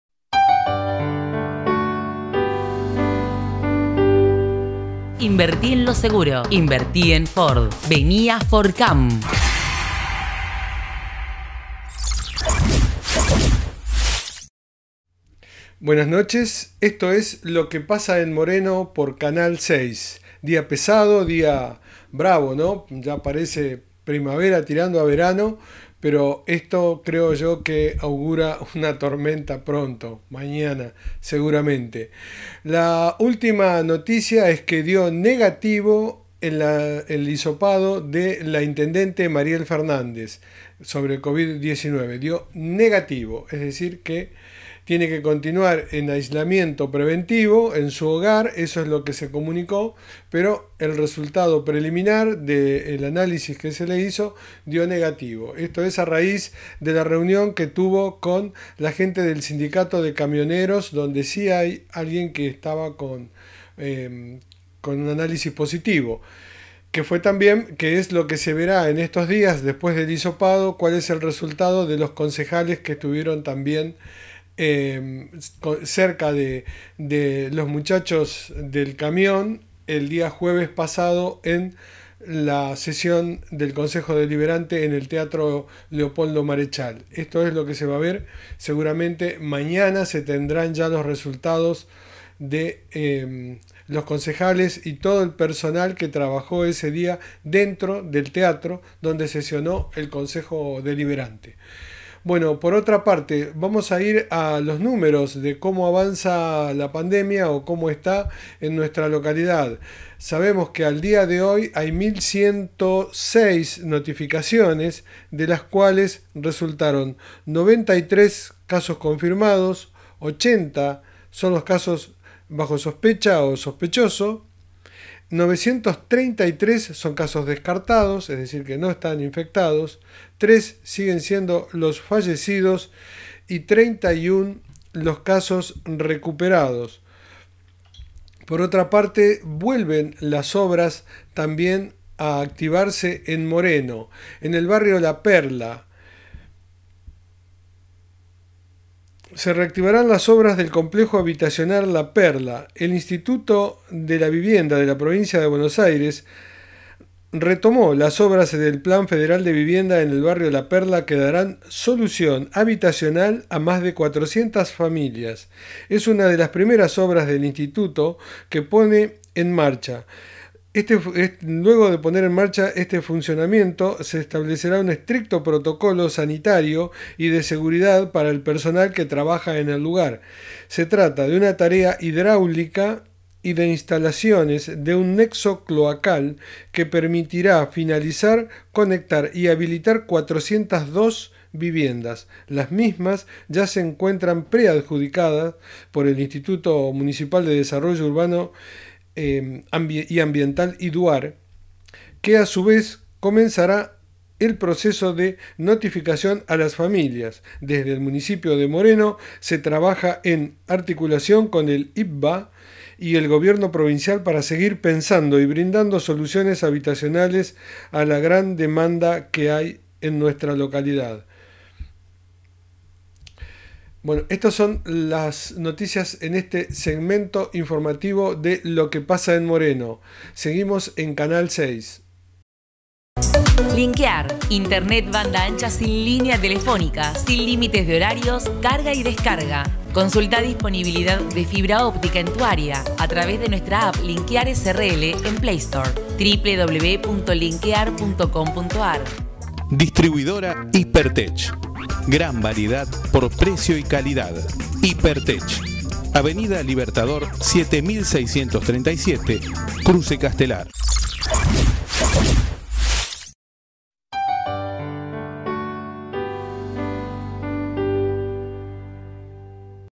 DIO NEGATIVO EL ANÁLISIS DE MARIEL FERNANDEZ
DIO NEGATIVO EL ANÁLISIS DE MARIEL FERNÁNDEZ
Tras la reunión de la intendente de Moreno con los sindicalistas del gremio de camioneros la semana pasada y luego de saberse que dio positivo el análisis sobre COVID-19, del secretario gremial Marcelo Aparicio. Mariel Fernández, también tuvo que someterse a análisis, con el objeto de descartar un posible contagio. Hoy desde el municipio de Moreno se informó que la intendente Mariel Fernández dio negativo, pero si debe seguir en aislamiento preventivo en su hogar hasta descartar por completo cualquier duda.
Se informo también que desde su domicilio seguirá la actividad diaria de la municipalidad.